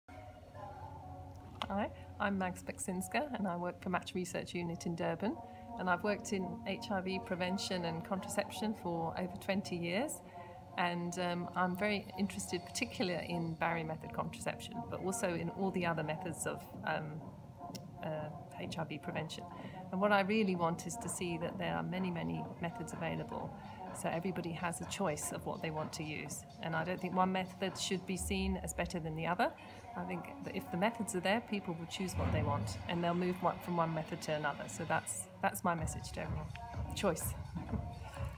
Dr. Mags Beksinska
Comments on the importance of choice in different methods of HIV prevention - SAHCS 2018 Conference